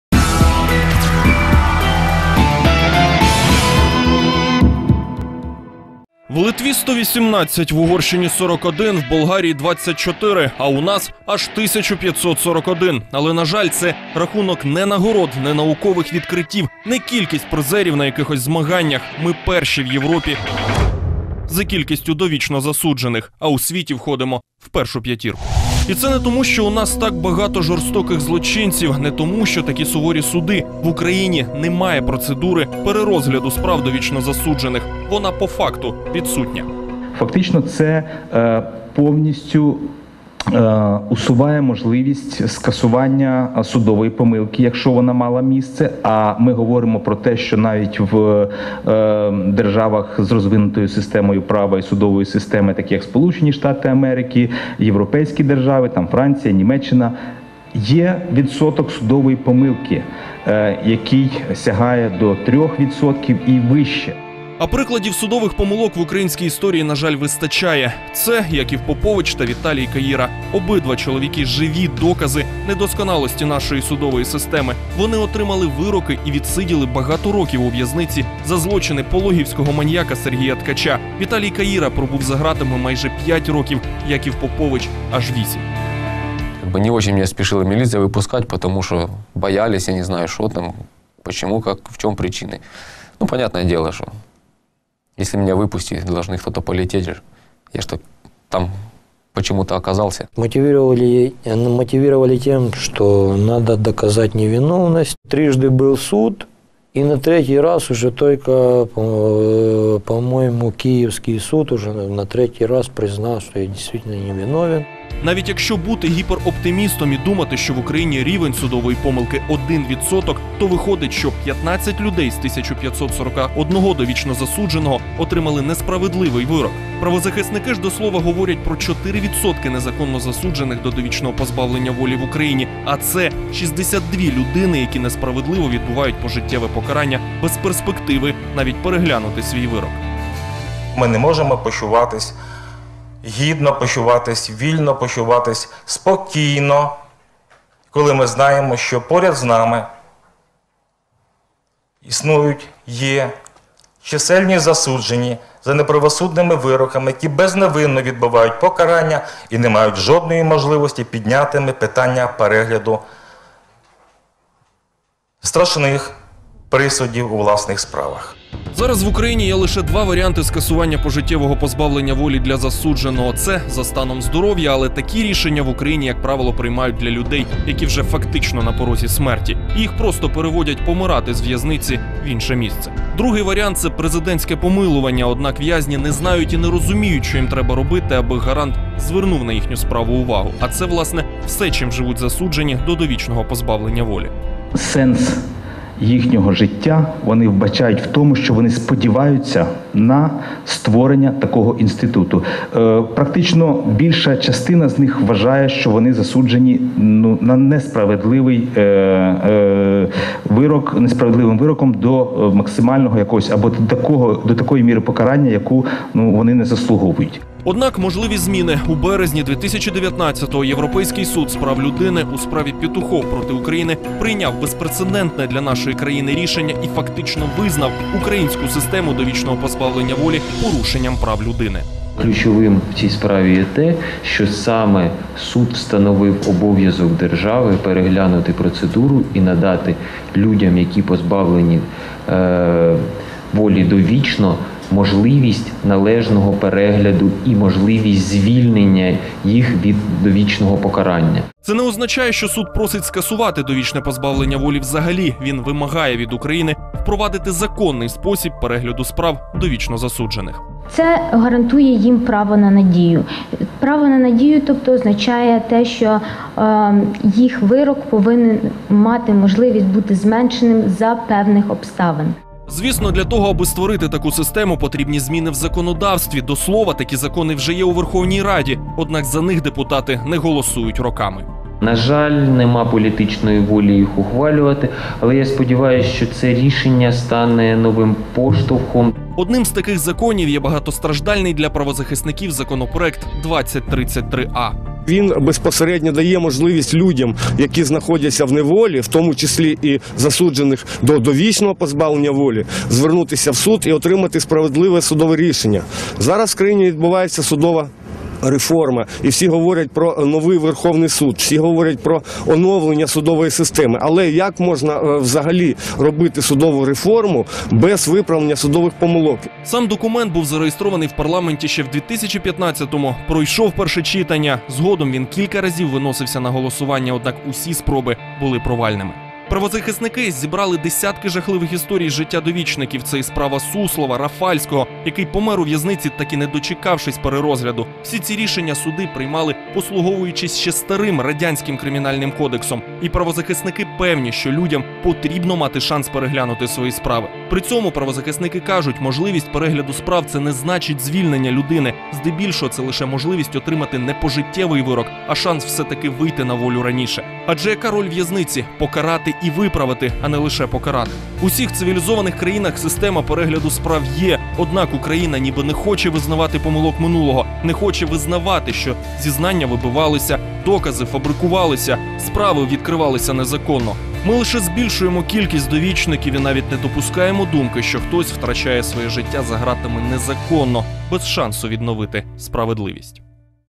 В Україні може з'явитися система перегляду вироків довічникам
Європейський суд з прав людини визнав українську систему довічного ув'язнення порушенням прав людини.